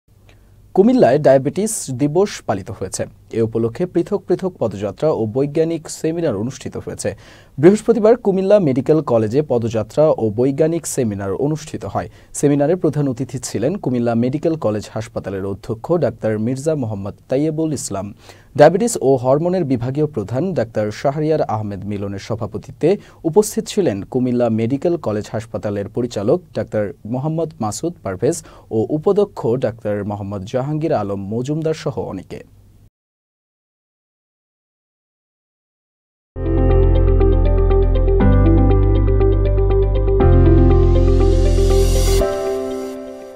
কুমিল্লায় ডায়াবেটিস দিবস উপলক্ষে পদযাত্রা ও বৈজ্ঞানিক সেমিনার অনুষ্ঠিত | Diabetes Day | Channel S
কুমিল্লায় ডায়াবেটিস দিবস উপলক্ষে পদযাত্রা ও বৈজ্ঞানিক সেমিনার অনুষ্ঠিত | Diabetes Day | Channel S News

1: News :
2: Entertainment News :
3: Sports News :